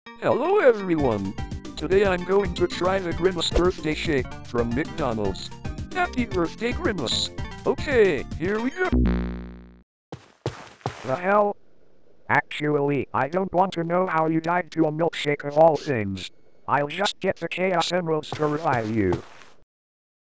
Sonic tries the Grimace Birthday Shake
Sonic tries the new Grimace Birthday Shake.

It... doesn't exactly go to plan.

Grimace Shake Sprite made by me